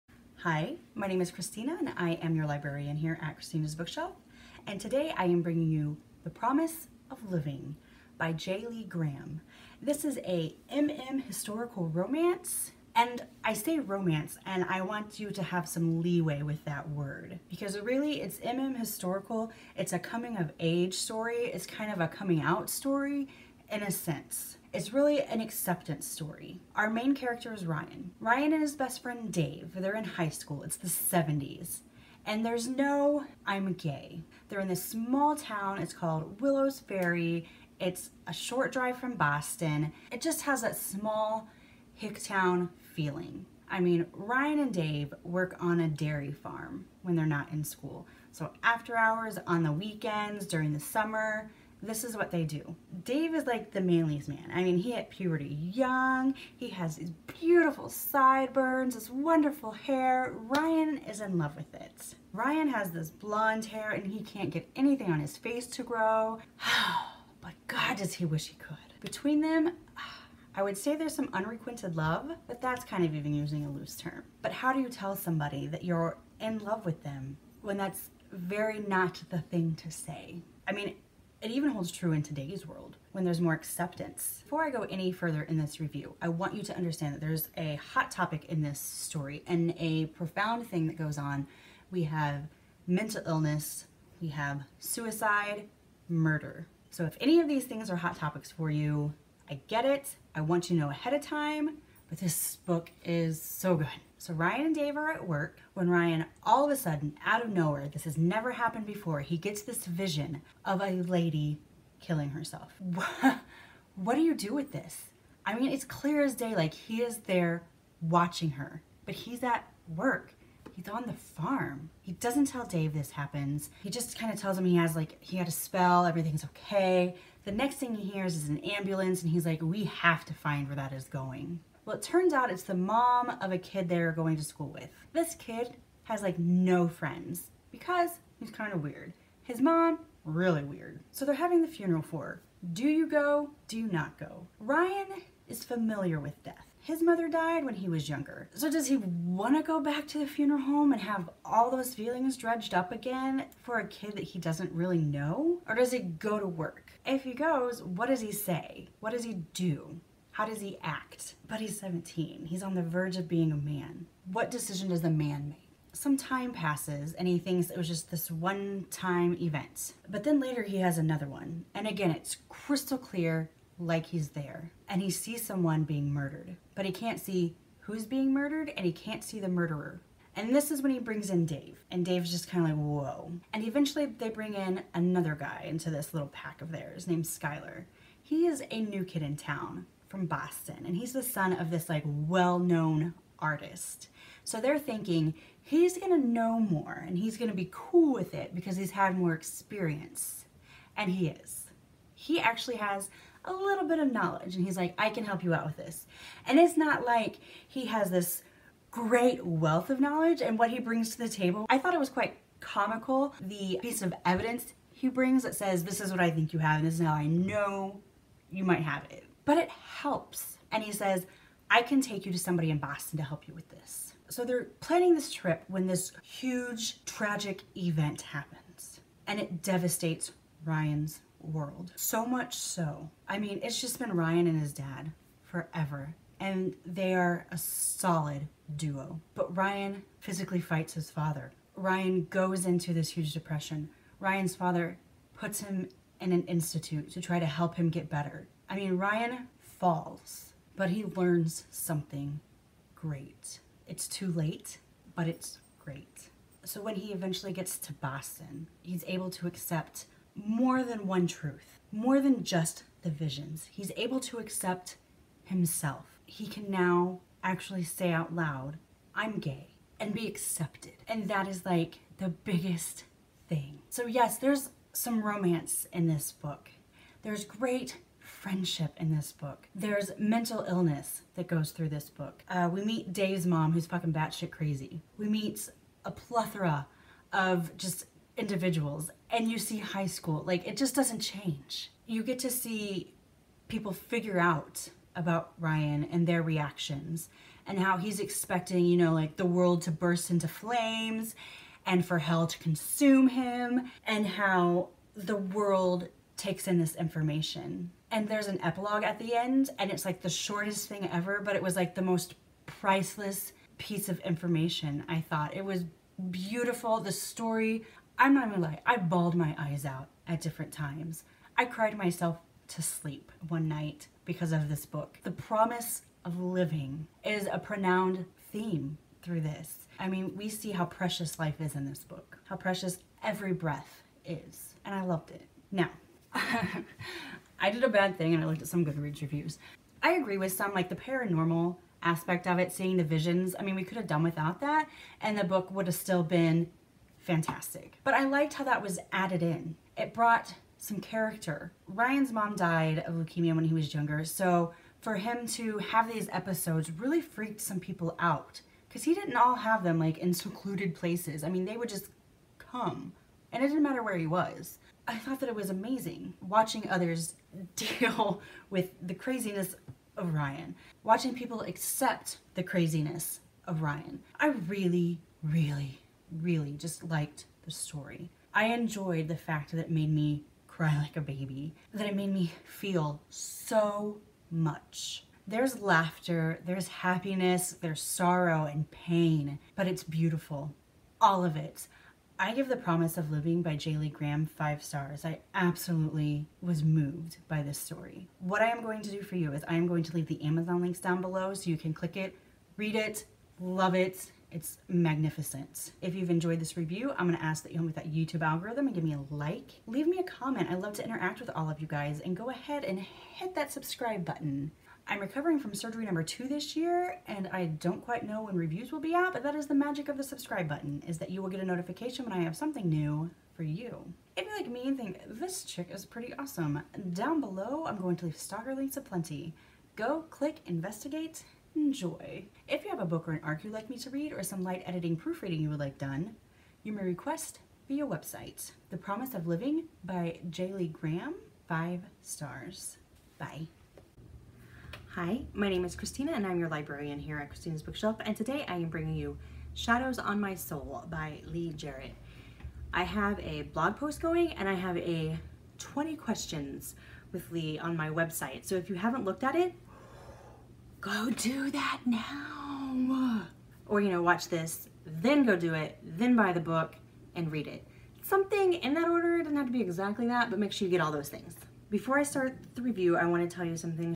The Promise of Living by J.Lee Graham
**This story mentions suicide, homicide, and mental illness.**

This is a coming of age story that takes place in the 70's.  Ryan and Dave are both juniors in high school getting ready to take over as seniors.  They've spent their lives preparing for this role.  Between school, the band, and working on a local dairy farm they dream about their future.  Only they keep some dreams to themselves.

One day Ryan is hit with a vision so clear of a woman committing suicide.  He's distraught by the image, it's clarity, the meaning.  Is it real?  When the visions continue he tells Dave.

Ryan and Dave then bring in a third person and together they try to figure exactly what is happening to Ryan.  They all understand that with such great power comes great responsibility.  If you don't understand it what can you do?  Especially if you see a murder happening but you don't know when, who is being murdered, or who is the murderer.

Then a tragedy strikes Ryan so deep he feels he'll never recover.  His whole world is turned upside down.

From here he finds the strength to finally accept who he is in more ways than one.